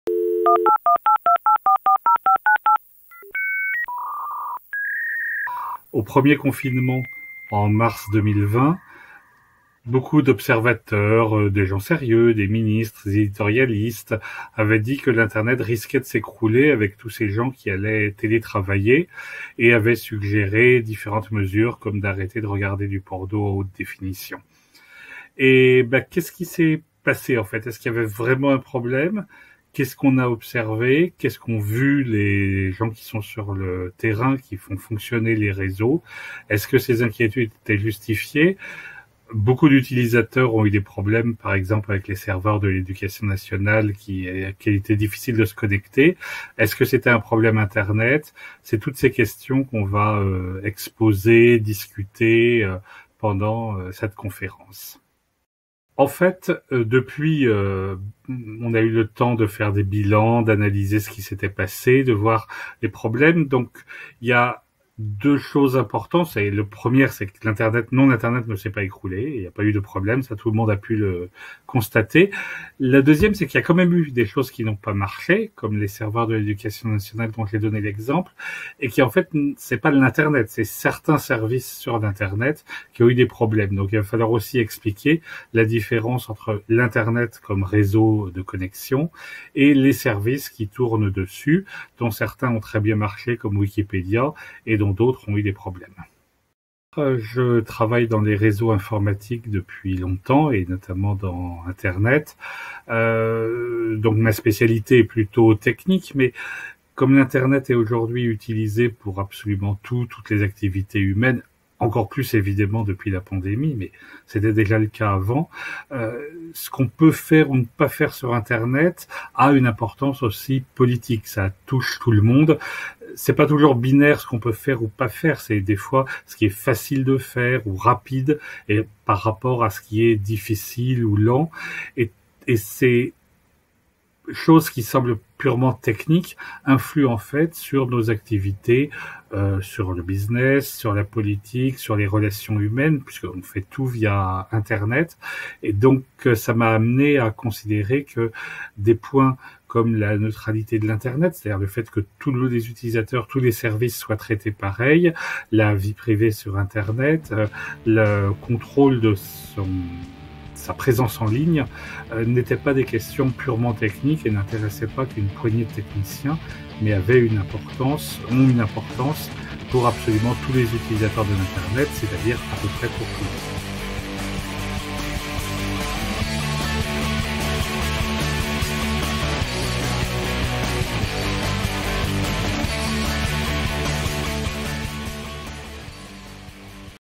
Stéphane Bortzmeyer, interview
Stéphane Bortzmeyer, ingénieur R&D à l’Association française pour le nommage Internet en coopération (Afnic), animera une conférence qui aura pour thème "Internet et Confinement" le mercredi 28 juillet lors d'Entrée Libre #2 organisé du 28 au 31 juillet 2021 par et au Centre des Abeilles, 4 jours de conférences et d'ateliers pour ne plus se sentir démuni·e avec le numérique